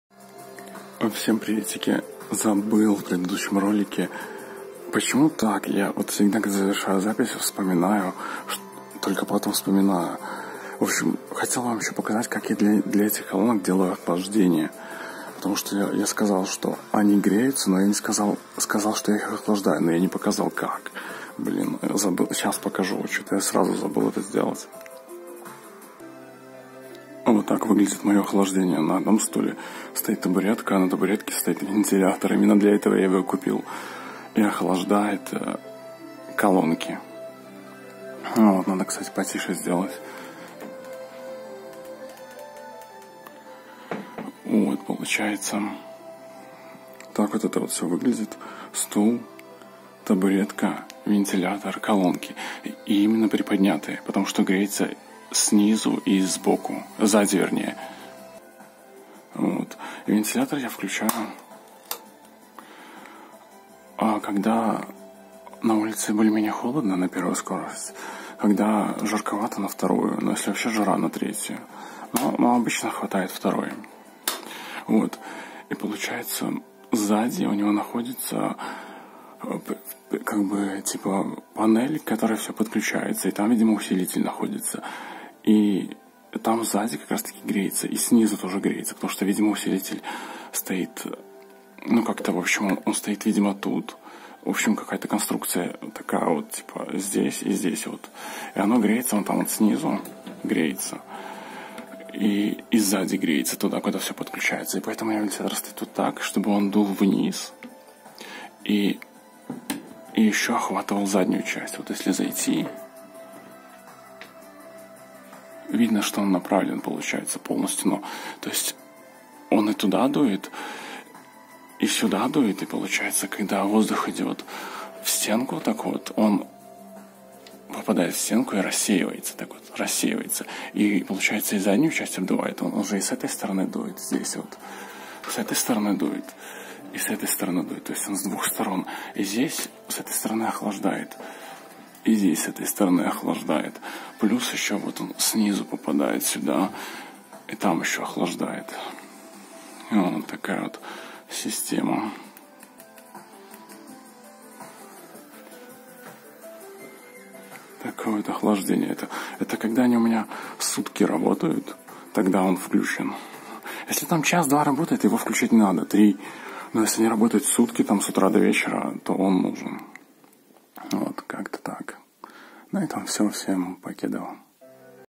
Microlab Solo 9c - дополнительное охлаждение
Эти колонки начинают греться когда долго работают. Поэтому я сделал для них дополнительное охлаждение. Ну если они у вас работают часа 3, то вы этого не заметите. Ну или если в момент прослушивания музыки вы ни разу не трогали заднюю панель, то тоже не заметите :)